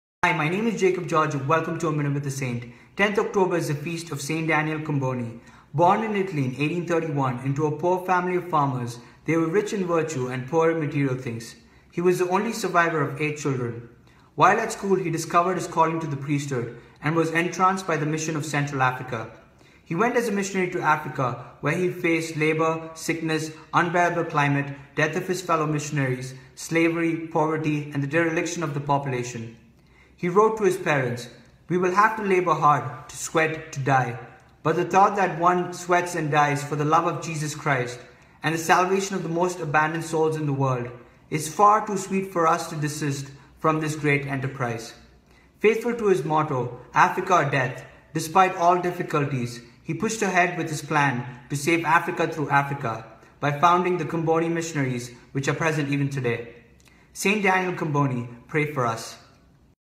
A Minute with a Saint - Feast of St. Daniel Comboni (10th October)
A Minute with a Saint!
Feast of St. Daniel Comboni (10th October)

“We will have to labour hard, to sweat, to die: but the thought that one sweats and dies for love of Jesus Christ and the salvation of the most abandoned souls in the world, is far too sweet for us to desist from this great enterprise” - St. Daniel Comboni